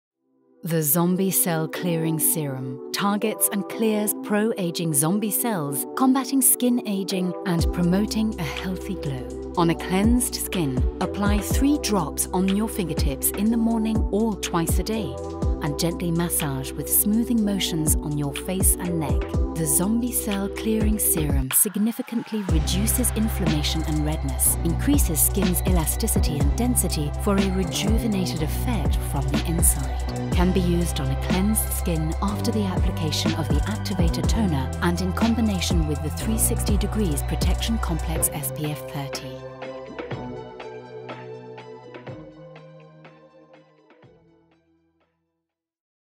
How to Apply your Zombie Cell Clearing Serum I Nescens Beauty
Follow this tutorial that will teach you the advanced application techniques of the Zombie Cell Clearing Serum in the unique style of Nescens.

Immerse yourself in our expertise to maximize the benefits and refine your skincare routine.

Benefits of the Zombie Cell Clearing Serum – Face :

- Reduction of redness
- Increase in elasticity
- Increase in dermis + epidermis density
- Gives the skin a healthy glow effect
- Gives the skin a fresher appearance
- Sensation of soft and smooth skin for an extended period